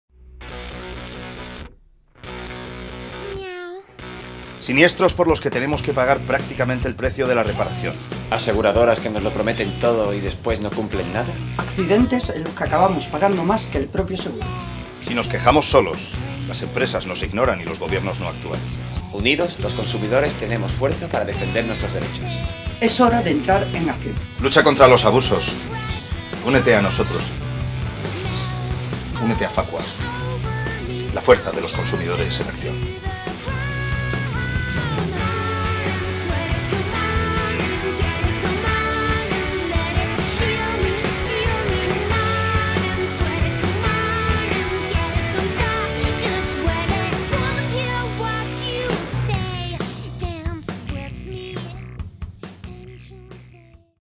Inseguros 3 - Lucha contra las aseguradoras que no cumplen con lo prometido
Versión protagonizada por Miguel Hermoso, con Álex O'Dogherty y Maica Barroso.

Siniestros por los que tenemos que pagar prácticamente el precio de la reparación. Aseguradoras que nos lo prometen todo y después no cumplen nada. Accidentes en los que acabamos pagando más que el propio seguro.

Lucha contra las aseguradoras que no cumplen con lo prometido

Lucha contra los abusos. Únete a nosotros. Únete a FACUA. La fuerza de los consumidores en acción.